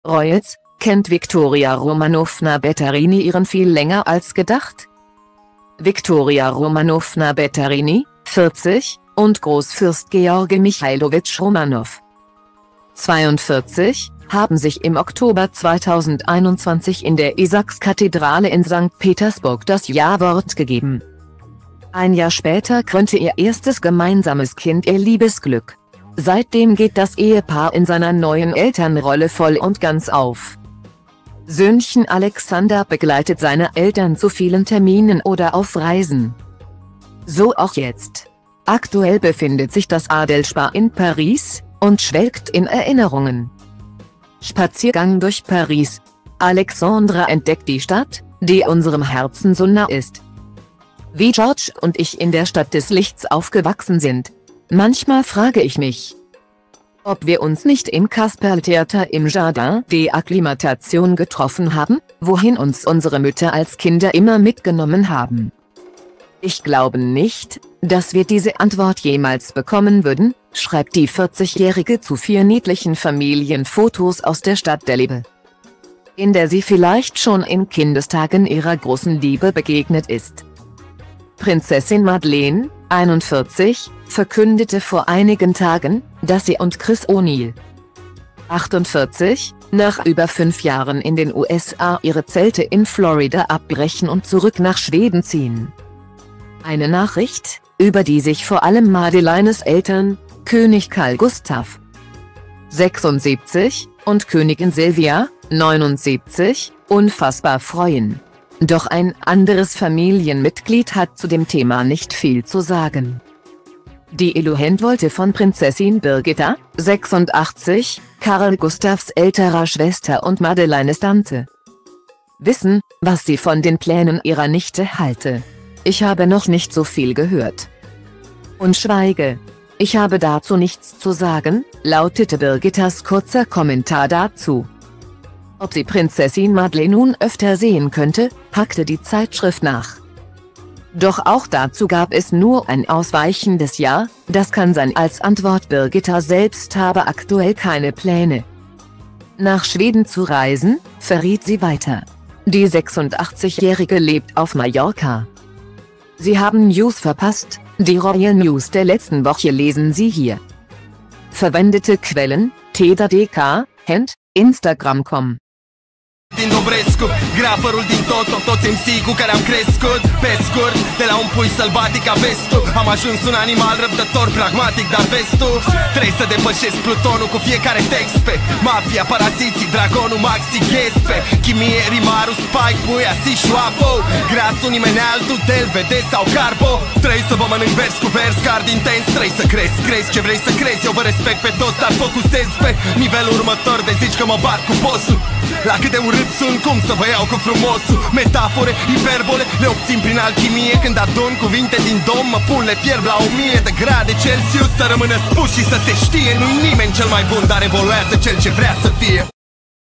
Royals: Kennt Victoria Romanowna Bettarini ihren viel länger als gedacht?
Victoria Romanowna Bettarini, 40, und Großfürst Georgi Michailowitsch Romanow,42, haben sich im Oktober 2021 in der Isaaks-Kathedrale in Sankt Petersburg das Jawort gegeben,ein Jahr später krönte ihr erstes gemeinsames Kind ihr Liebesglück. Seitdem geht das Ehepaar in seiner neuen Elternrolle voll und ganz auf,